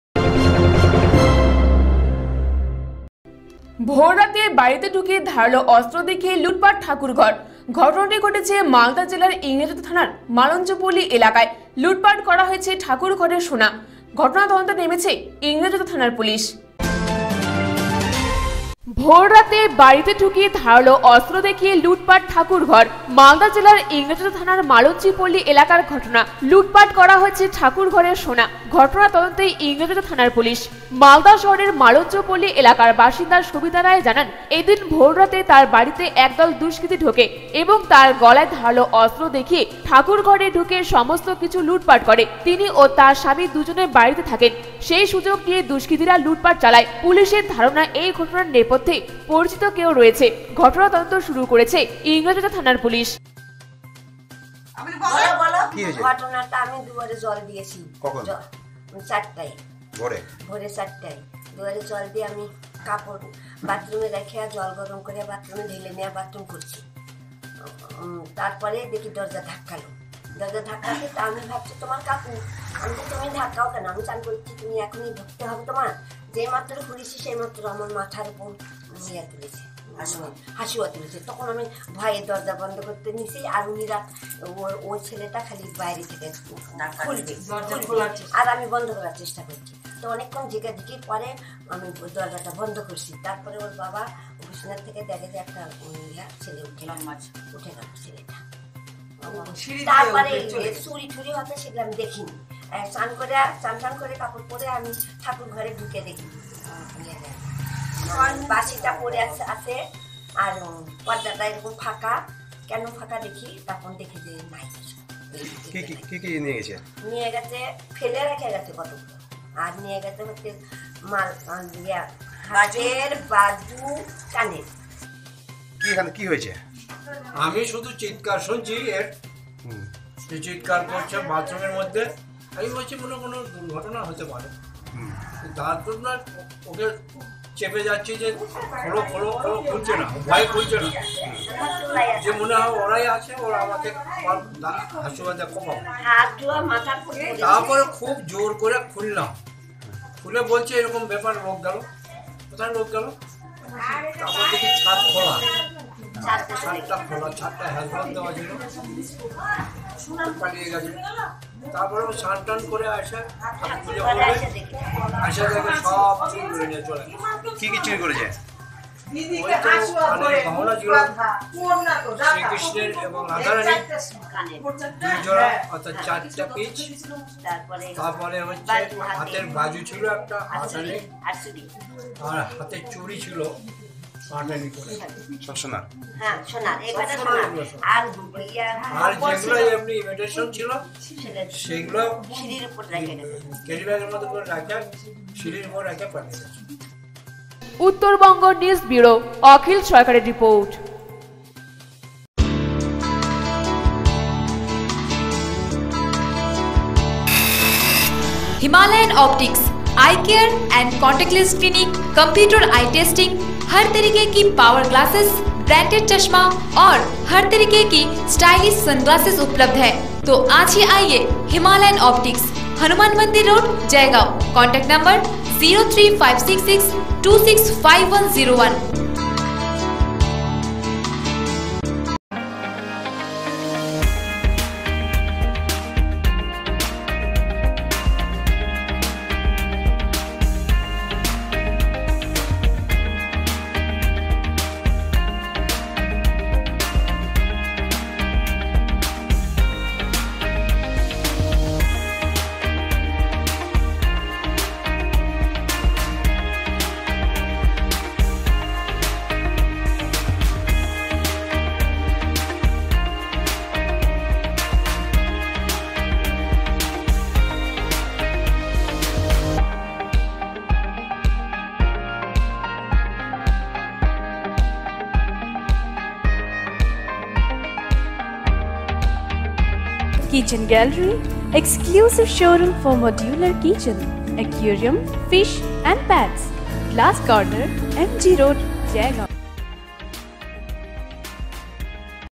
ভোর রাতে বাড়িতে ঢুকে ধারালো অস্ত্র দেখিয়ে লুটপাট ঠাকুর ঘরের মালদা জেলার ইংরেজবাজার থানার মালঞ্চ পল
ভোর রাতে বাড়িতে ঢুকে ধারালো অস্ত্র দেখিয়ে লুটপাট ঠাকুর ঘরের মালদা জেলার ইংরেজবাজার থানার মালঞ্চ পল্লী এলাকার ঘটনা